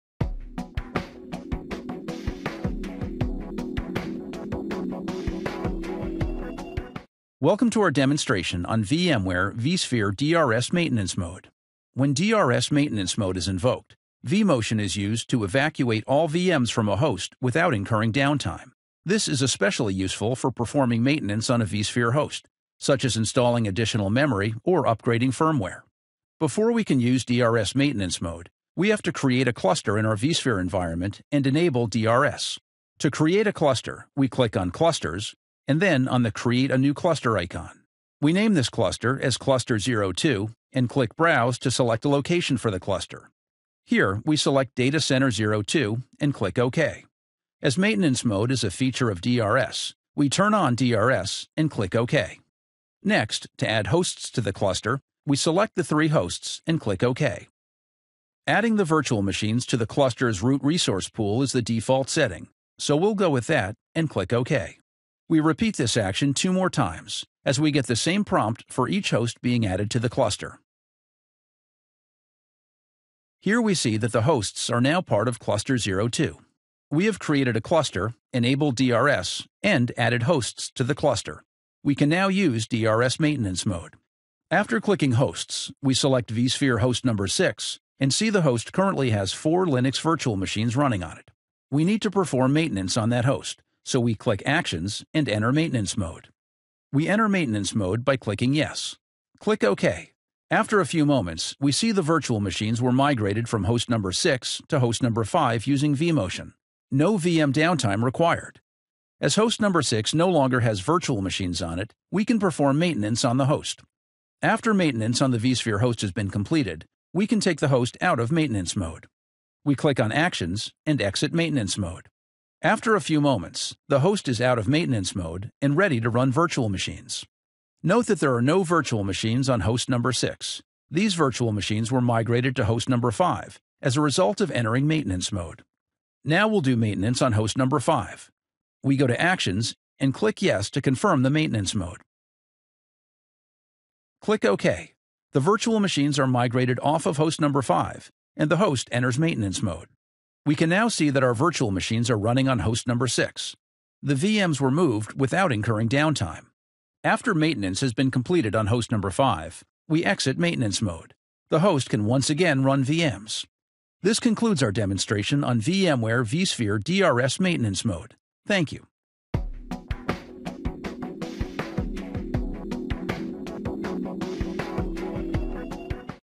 Enable Host Maintenance Mode for VMware vSphere (vSOM)
This video shows how to use VMware vSphere Maintenance Mode to automate the task of migrate virtual machine workloads off vSphere hosts in preparation for host maintenance with vSOM.